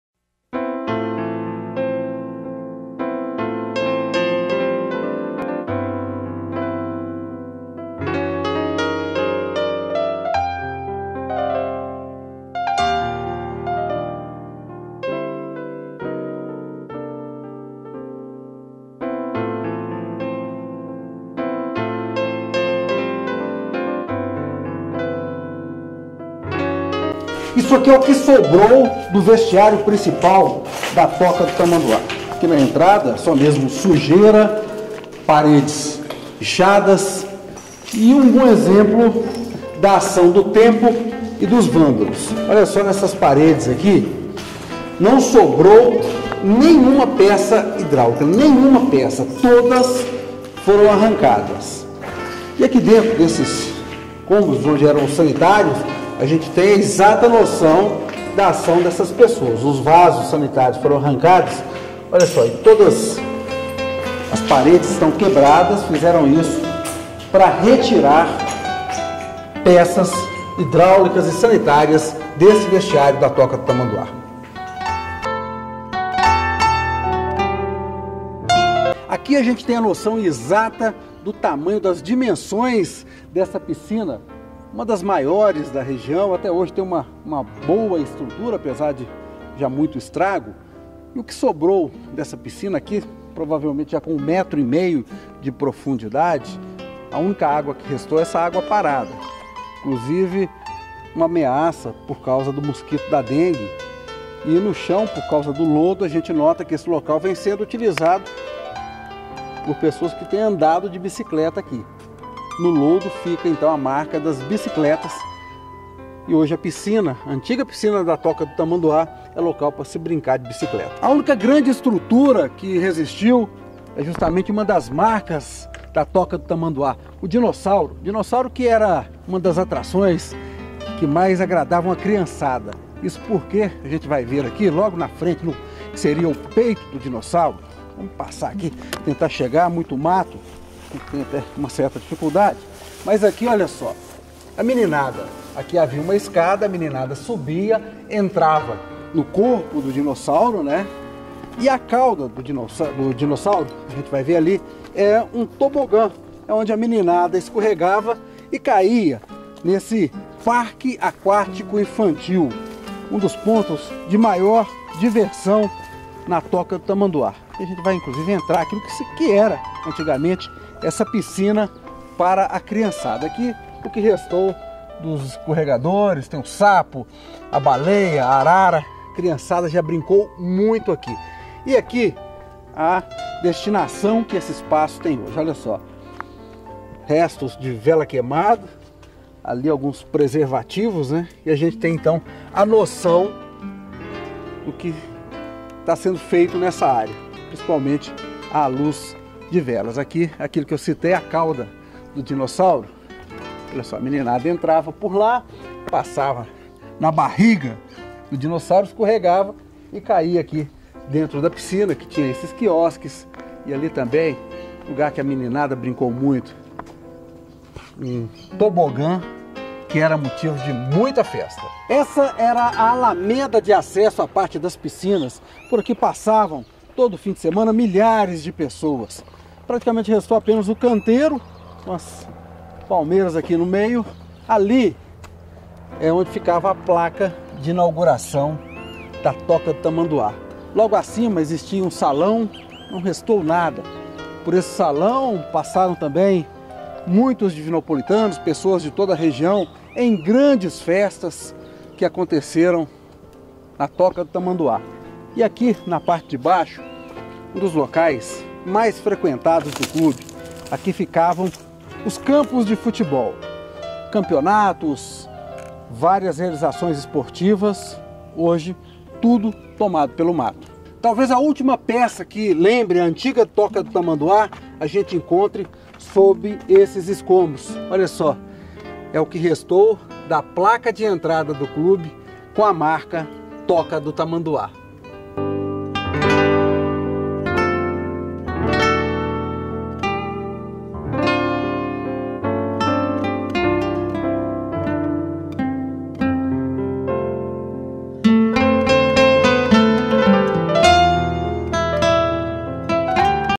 Toca do Tamanduá: só restou a saudade para contar a história (reportagem de 2015)
Na primeira metade da década de 1990, não havia Estrela do Oeste Clube, Divinópolis Clube, Iate, Havaí, CSM ou Art (telemig) que chegasse aos pés da Toca do Tamanduá em animação e movimento. Finais de semana eram intensos no clube, que funcionava no Bairro Santos Drummond. Mas, aos poucos, tudo foi se acabando. O badalado salão de festas, a piscina olímpica, os dinossauros das piscinas infantis, as churrasqueiras e os campos de futebol. Ah, esses eram o coração do clube. Quando voltei lá para essa reportagem, em 2015, já não havia quase nada. Poucas coisas ainda rememoravam o passado glorioso. Hoje, nem isso mais há. O terreno foi arrematado em um leilão para quitar dívidas e hoje pertence a particulares.